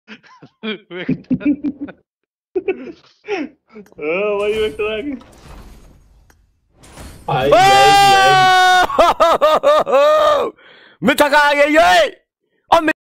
MRJAY PLAYS PAU C METHIC NIKALTY HUVY
Mr jay plays ny pau c methic nikala :P new trick ijad kerty huvy jis jis ko methic nikalna he wo apny pau k anguty c mouse ko click ker ke methac nikal skta he :P